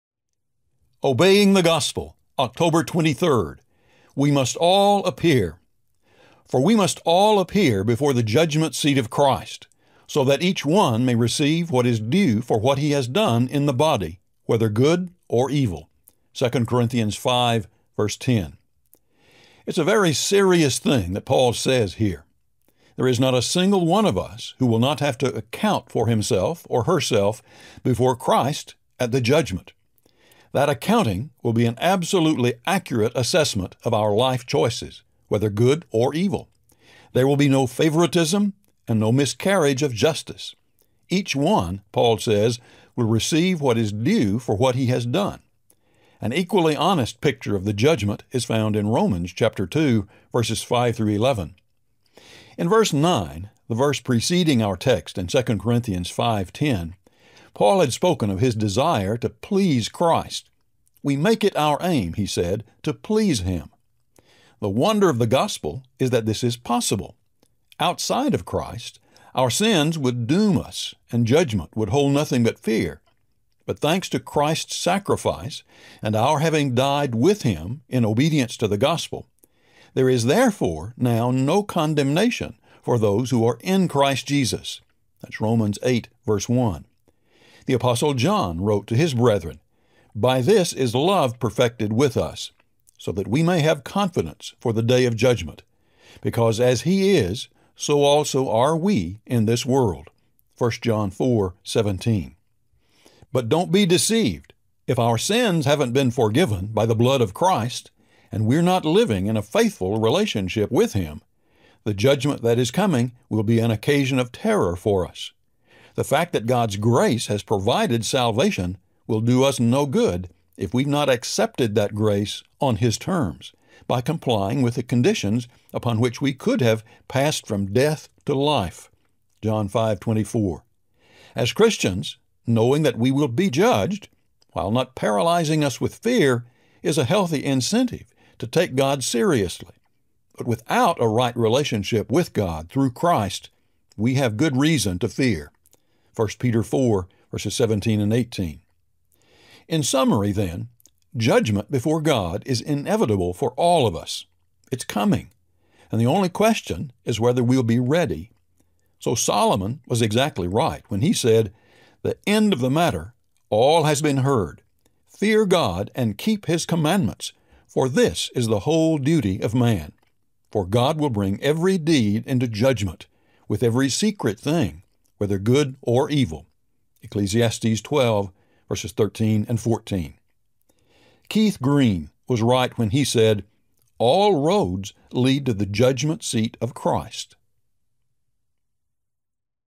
We Must All Appear (October 23)
"For we must all appear before the judgment seat of Christ so that each one may receive what is due for what he has done in the body, whether good or evil" (2 Corinthians 5:10).

IT IS A VERY SERIOUS THING THAT PAUL SAYS HERE. There is not a single one of us who will not have to account for himself or herself before Christ at the judgment. That accounting will be an absolutely accurate assessment of our life-choices, whether good or evil. There will be no favoritism and no miscarriage of justice. Each one will “receive what is due for what he has done.” An equally honest picture of the judgment is found in Romans 2:5-11.

In v.9, the verse preceding our text in 2 Corinthians 5:10, Paul had spoken of his desire to please Christ: “we make it our aim to please him.” The wonder of the gospel is that this is possible. Outside of Christ, our sins would doom us and judgment would hold nothing but fear, but thanks to Christ’s sacrifice and our having died with Him in obedience to the gospel, “There is therefore now no condemnation for those who are in Christ Jesus” (Romans 8:1). The apostle John wrote to his brethren, “By this is love perfected with us, so that we may have confidence for the day of judgment, because as he is so also are we in this world” (1 John 4:17).

But don’t be deceived: if our sins haven’t been forgiven by the blood of Christ and we’re not living in a faithful relationship with Him, the judgment that is coming will be an occasion of terror for us. The fact that God’s grace has provided salvation will do us no good if we have not accepted that grace on His terms, by complying with the conditions upon which we could have “passed from death to life” (John 5:24). As Christians, knowing that we will be judged, while not paralyzing us with fear, is a healthy incentive to take God seriously. But without a right relationship with God through Christ, we have good reason to fear (1 Peter 4:17,18).

In summary, then, judgment before God is inevitable for all of us. It is coming — and the only question is whether we’ll be ready. So Solomon was exactly right: “The end of the matter; all has been heard. Fear God and keep his commandments, for this is the whole duty of man. For God will bring every deed into judgment, with every secret thing, whether good or evil” (Ecclesiastes 12:13,14).

"All roads lead to the judgment seat of Christ" (Keith Green).